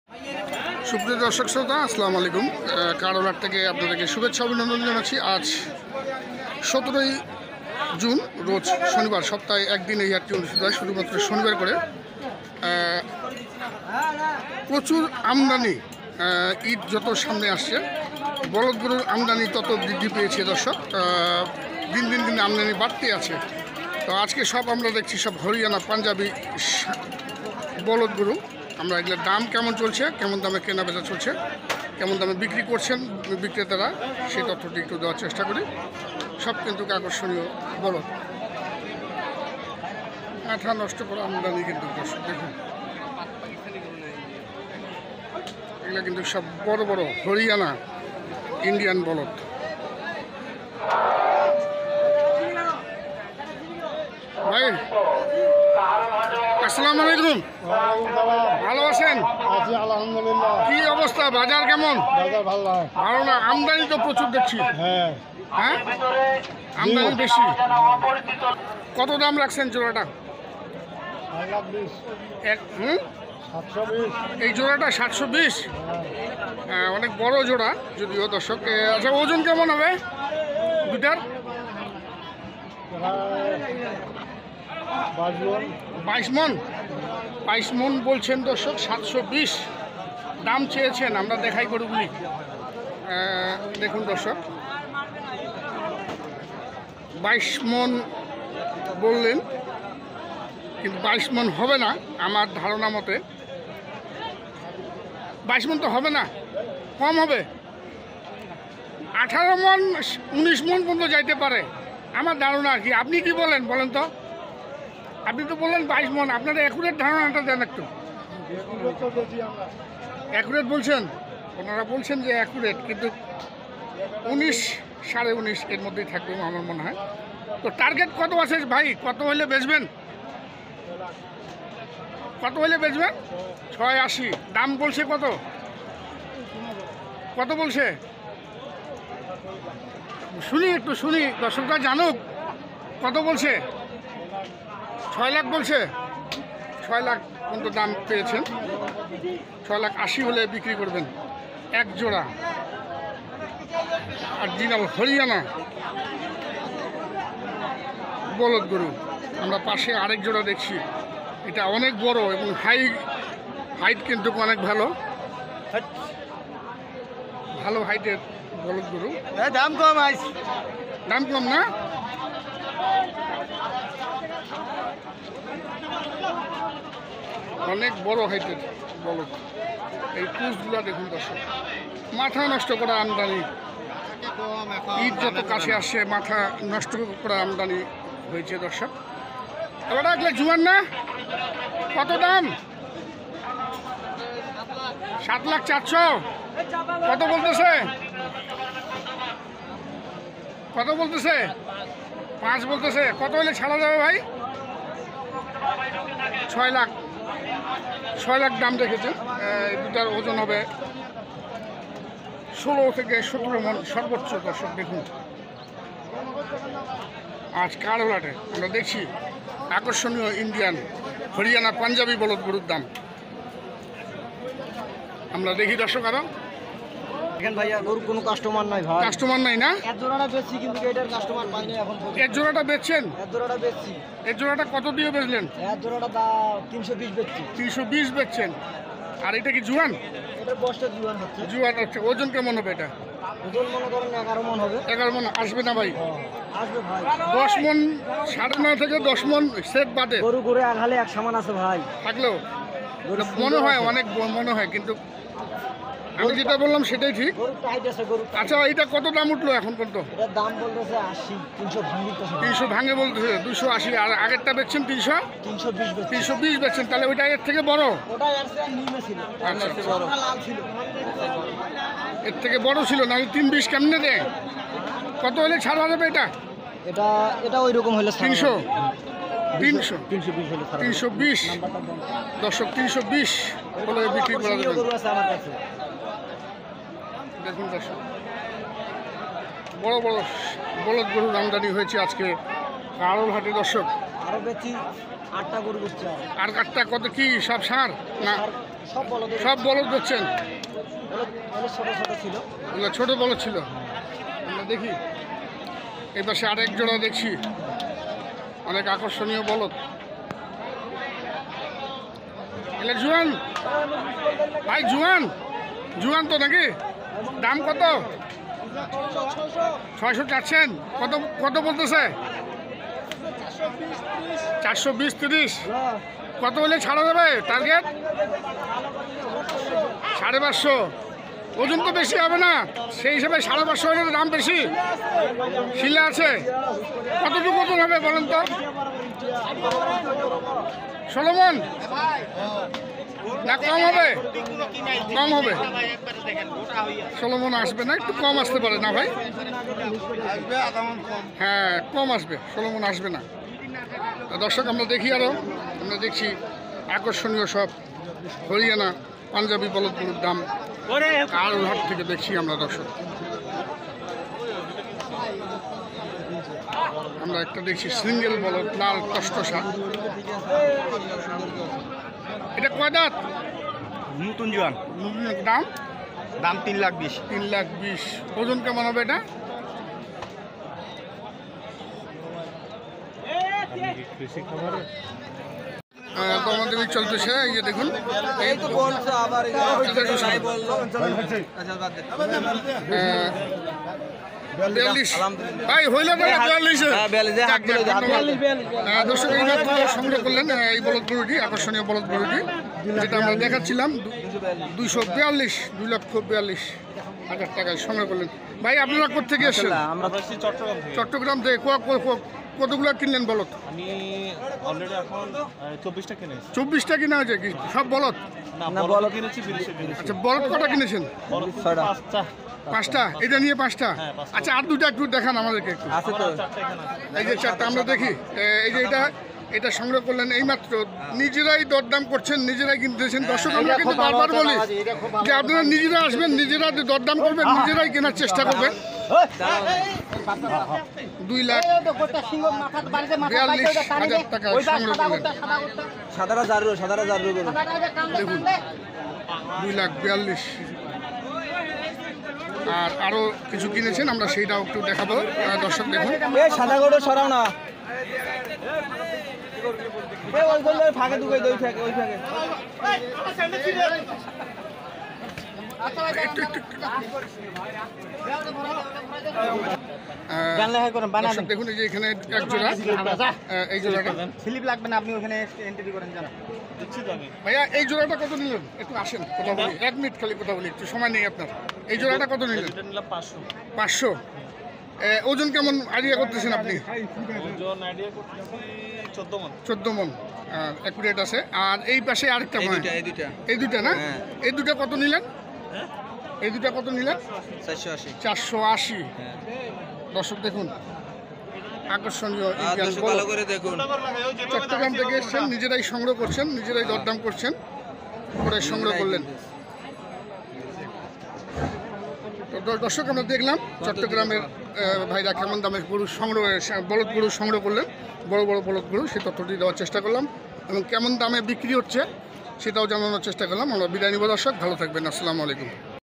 মাথা নষ্ট করা আমদানি ইন্ডিয়ান হরিয়ানা বলদ গরুর।। চট্টগ্রামের খামরী কি দামে বলদ ক্রয় করলো। কাহারোল।।
কাহারোল হাট।।